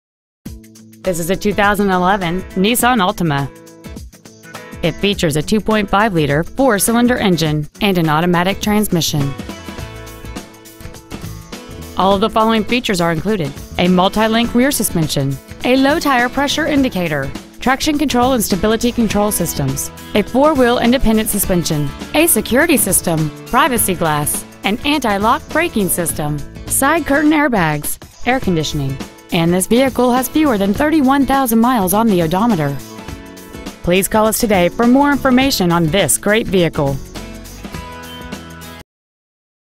Preowned 2011 NISSAN ALTIMA Chandler AZ
Year : 2011
 Make : NISSAN
 Model : ALTIMA
 Engine : 2.5L I4 16V MPFI DOHC
 Trans . : AUTOMATIC CVT
 Exterior : SILVER
 Miles : 30,025
 Interior : GRAY
 Stock : P54365